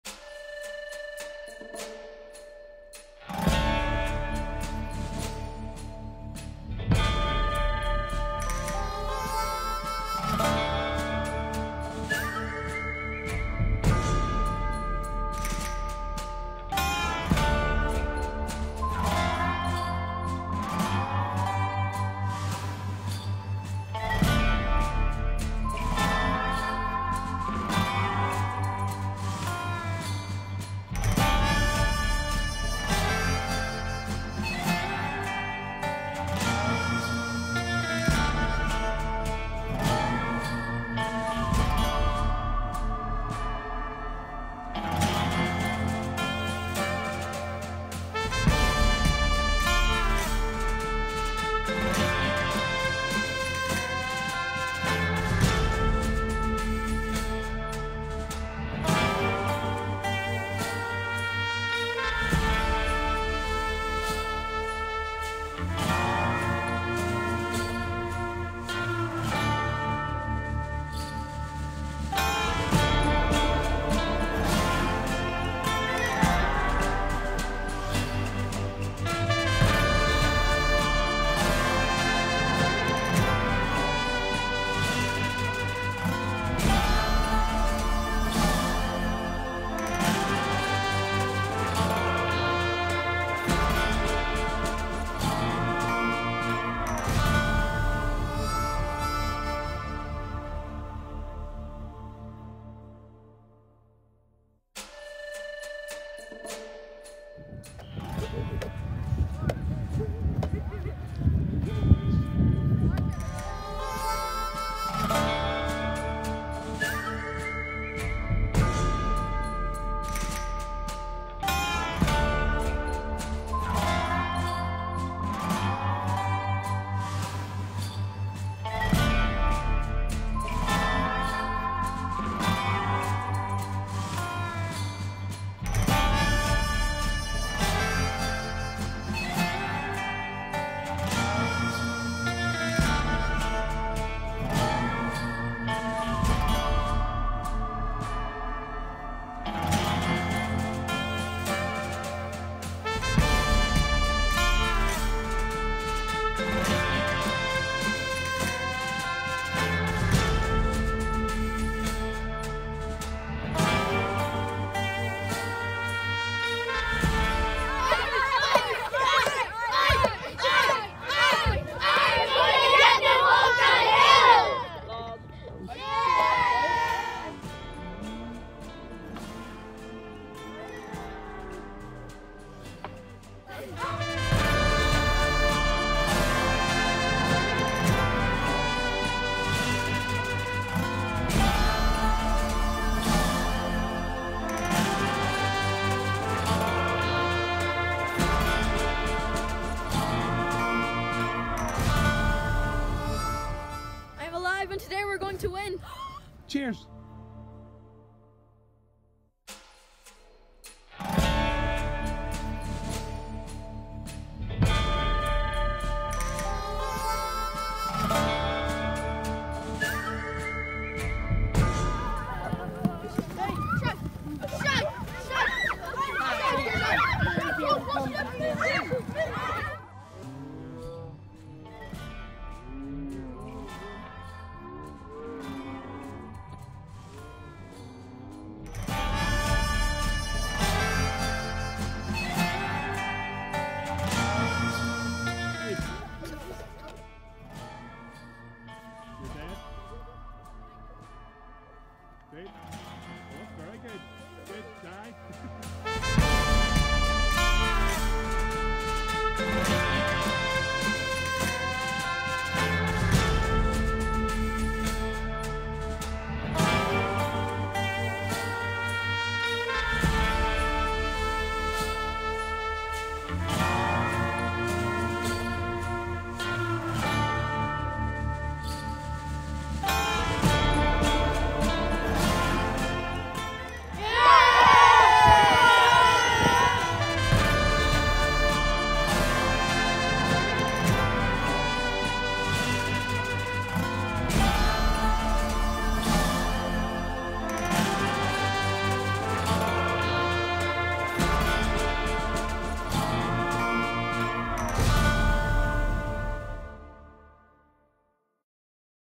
THE BATTLE OF HASTINGS (Class version)
This little video retells some of the key parts of the Battle of Hastings as performed by Year 8 :-)